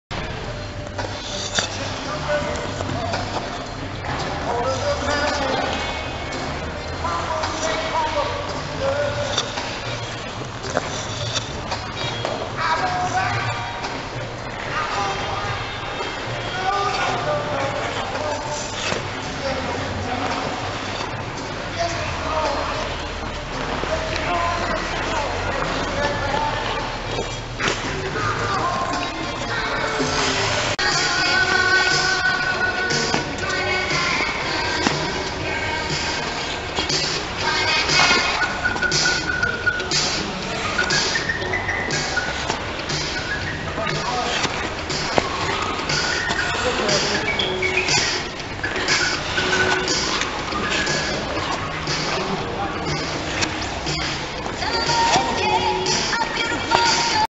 croisé avant amélioré
J essais d améliorer mon croisé avant, soyez pas trop dur, je fais de mon mieux avec mon handicap, j ai superposé deux vidéos,j espére que ça vous plaira